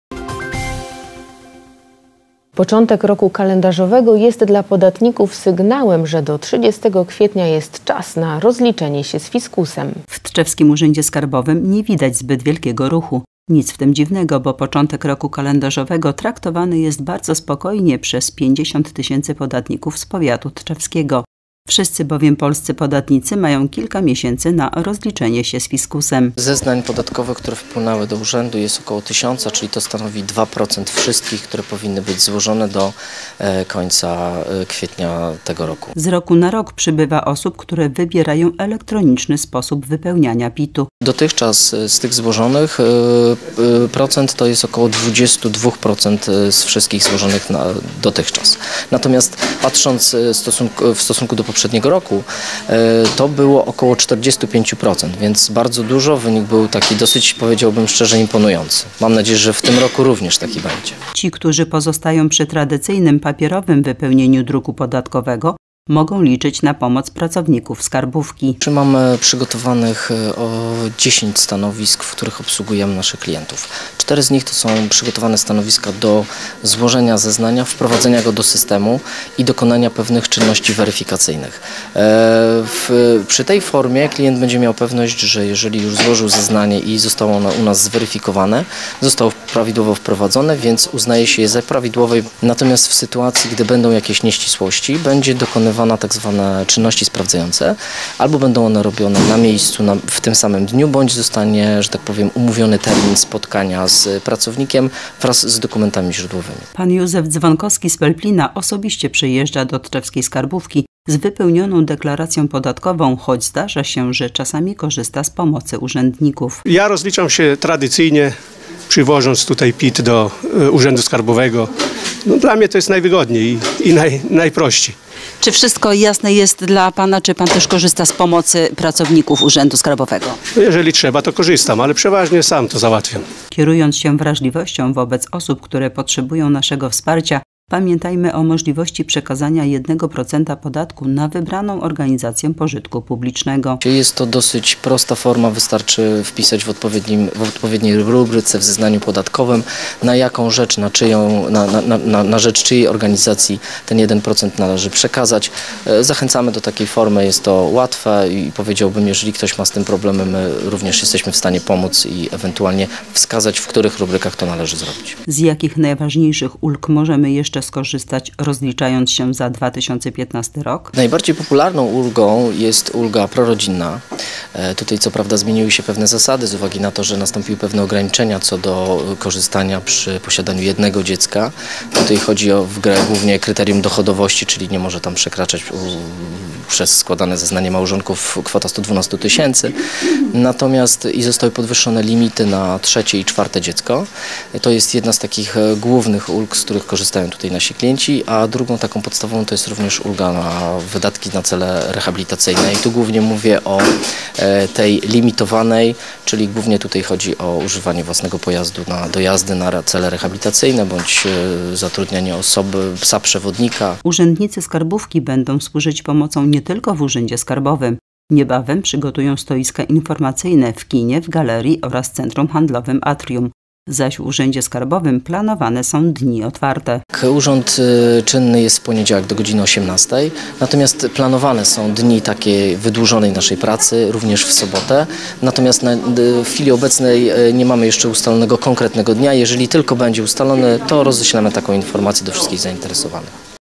PIT-y czekają na wypełnienie - Tv Tetka Tczew HD
Początek roku kalendarzowego jest zwiastunem dla podatników, którzy do 30 kwietnia muszą rozliczyć się z fiskusem.